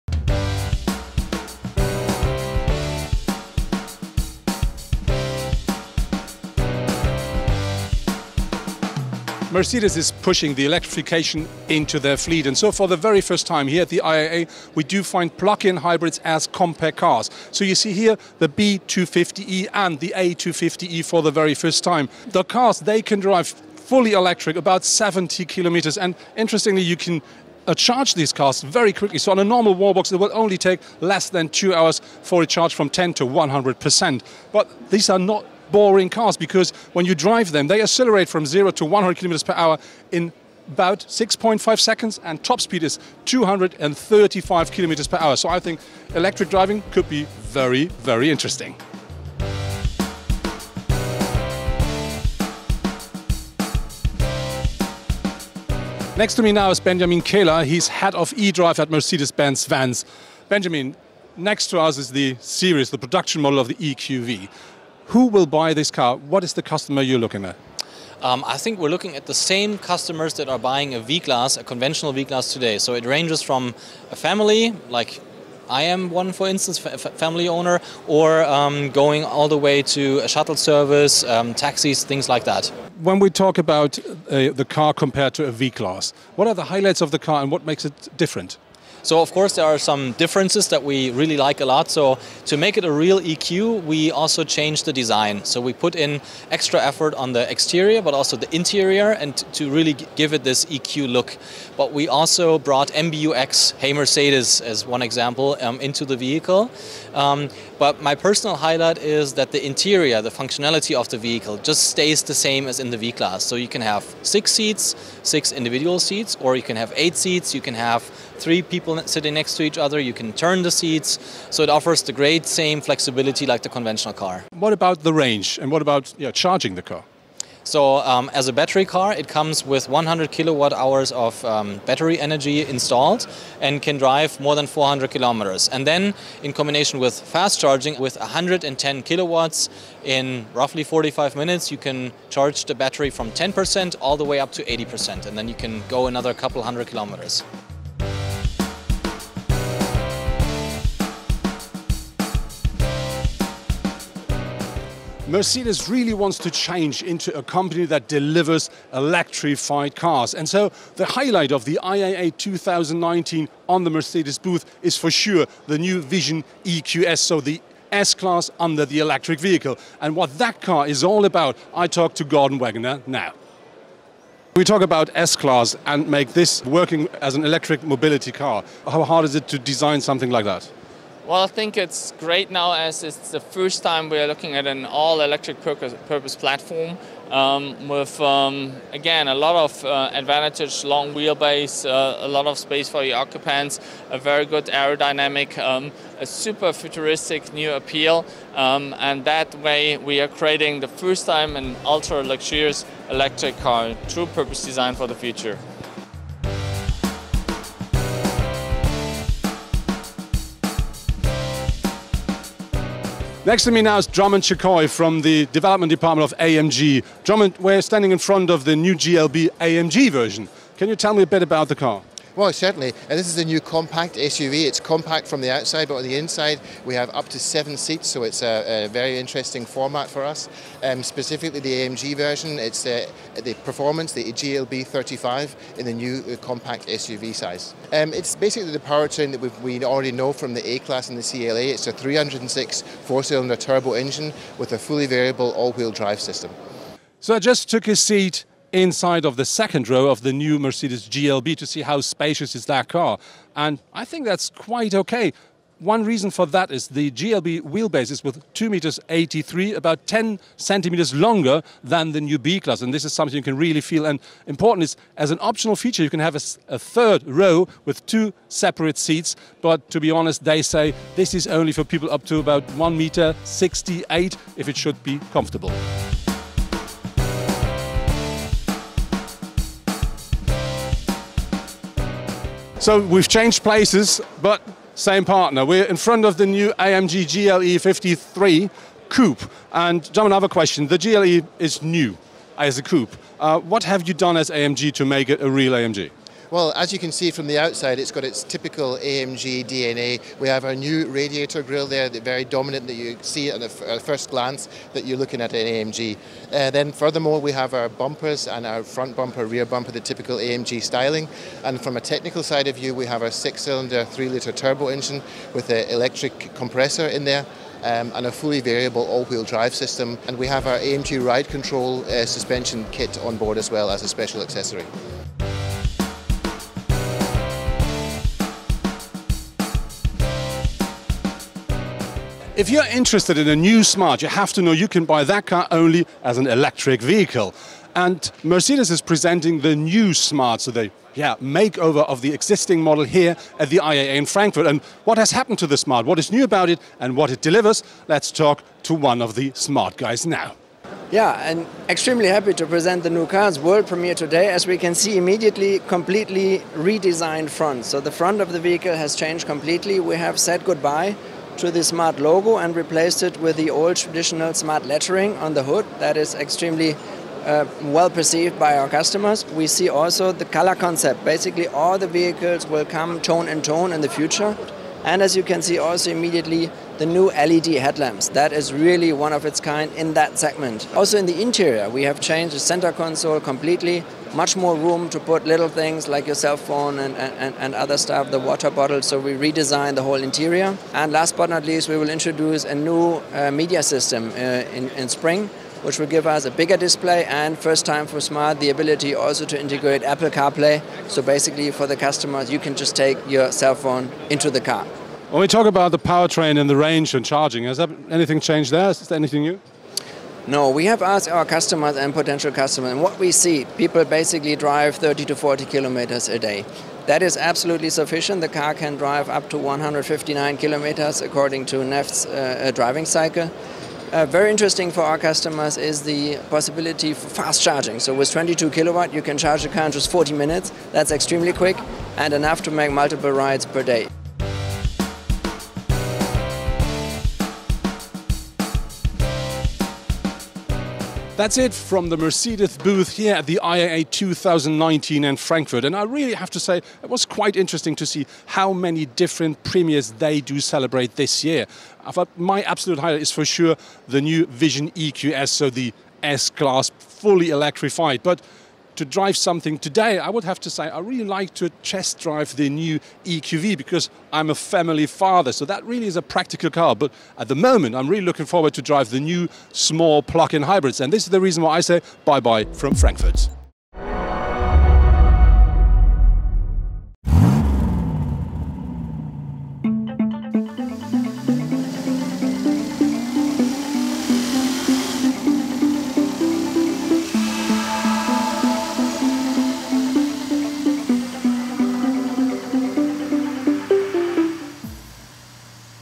Mercedes EQS, EQV and more | Smart EQ ForTwo | Motorvision
Lars Hoenkhaus visited the 2019 Frankfurt auto show and brought back information on Mercedes' fleet as well as the Smart EQ ForTwo.

TRY US FOR FREE ON AMAZON CHANNELS
Click this link and find out more about our new streaming service on Amazon Channels with exclusive shows!